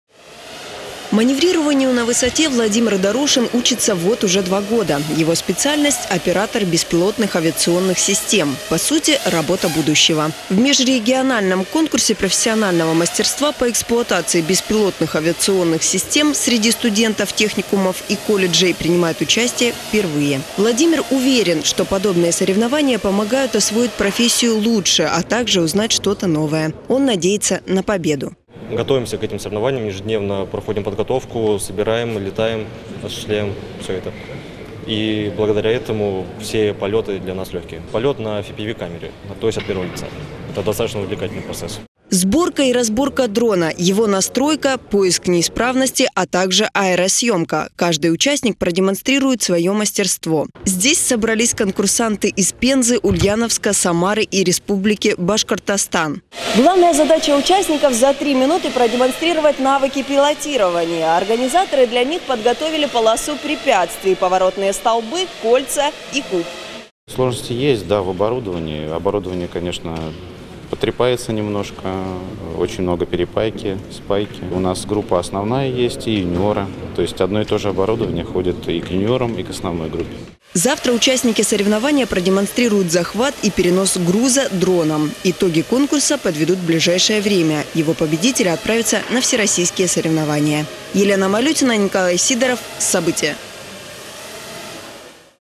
В Самаре прошли первые соревнования по управлению летающими дронами
Отчего люди не летают, как птицы - когда-то этим вопросом в своем произведении задавался Александр Островский, а сейчас современные технологии позволяют человеку смотреть с высоты птичьего полета. В Самарском техникуме авиационного и промышленного машиностроения им. Д.И. Козлова проходит межрегиональный конкурс профессионального мастерства по управлению летающими дронами. Участники съехались из 4 регионов России.  Подробнее в сюжете Елена Малютиной.